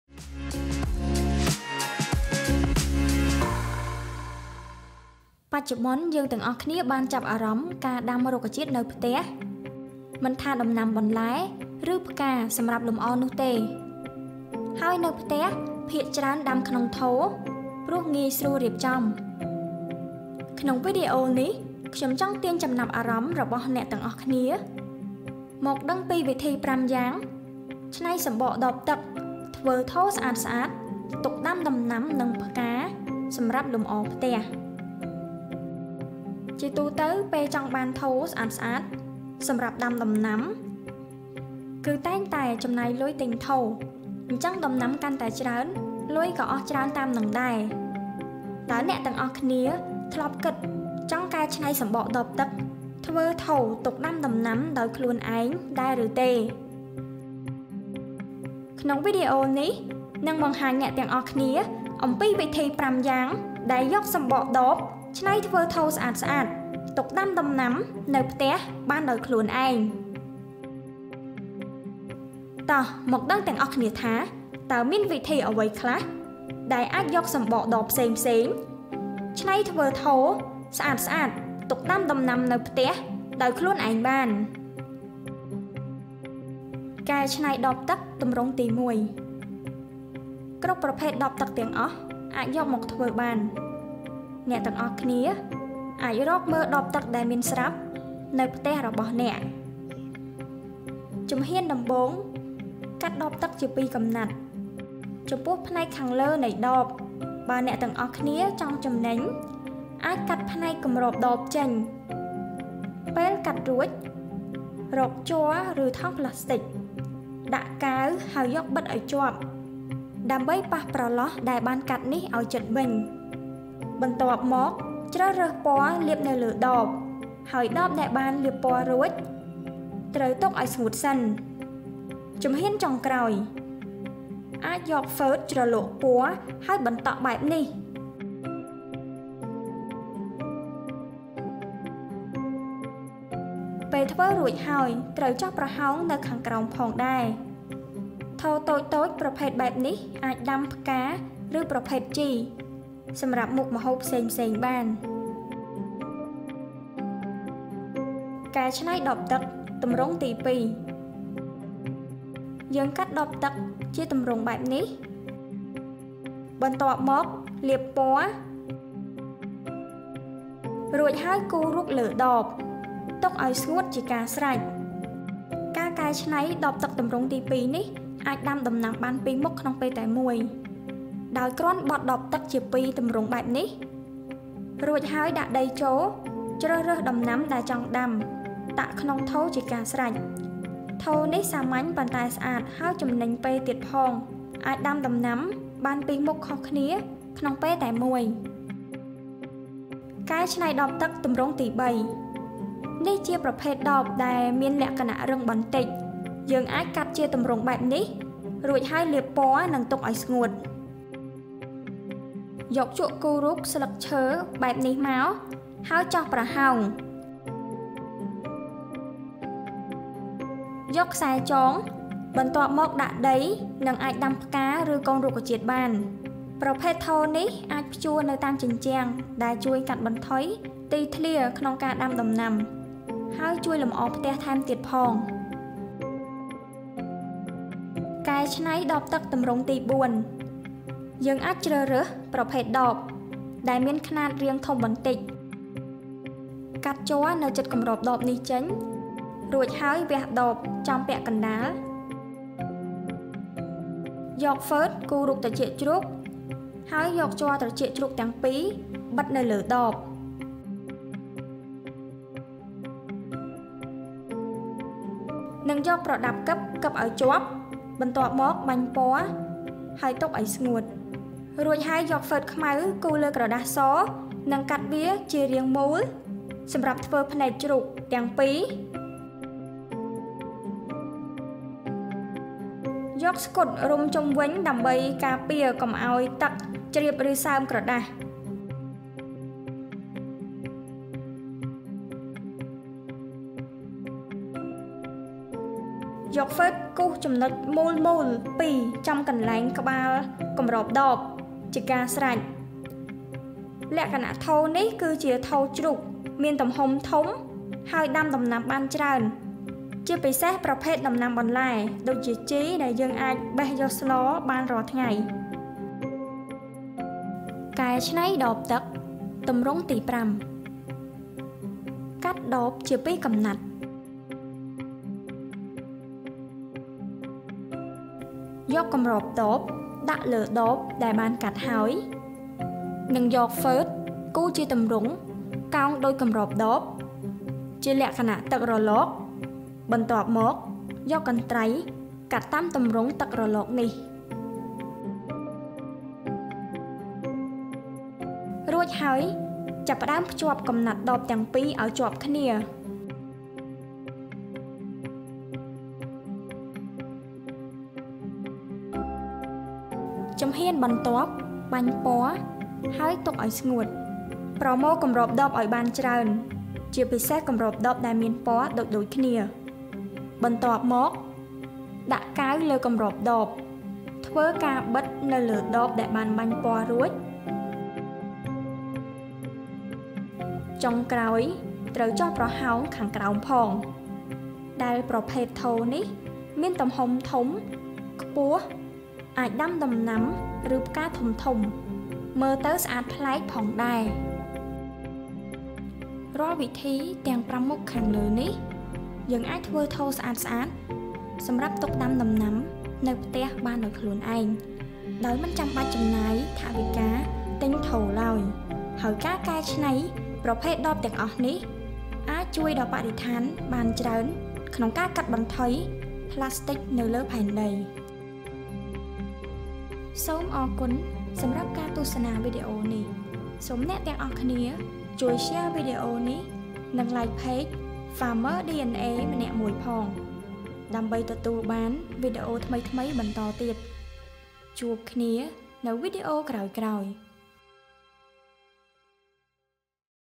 Agriculture Cambodia EP:08 ៥វិធី ច្នៃរសំបកដបទឹក ធ្វើថូ ទុកដាំដំណាំនៅផ្ទះបាន
5 Easy ways to recycle plastic bottles for plants grow at home
៥វិធី ច្នៃរសំបកដបទឹក ធ្វើថូ ទុកដាំដំណាំនៅផ្ទះបាន 🌽🥦🥒🥑

បច្ចុប្បន្ន យើងទាំងអស់គ្នាបានចាប់អារម្មណ៍ ការដាំរុក្ខជាតិនៅផ្ទះ
មិនថាដាំដំណាំបន្លែ រឺផ្កាសម្រាប់លម្អនោះទេ ហើយនៅផ្ទះភាគច្រើន
ដាំក្នុងថូ ព្រោះងាយស្រួលរៀបចំ។

ក្នុងវីដេអូនេះ ខ្ញុំចង់ទាញចំណាប់អារម្មណ៍ របស់អ្នកទាំងអស់គ្នា មកដឹងពី វិធី៥យ៉ាងច្នៃសំបកដបទឹក ធ្វើថូស្អាតៗទុកដាំដំណាំ និងផ្កា សម្រាប់លម្អផ្ទះ។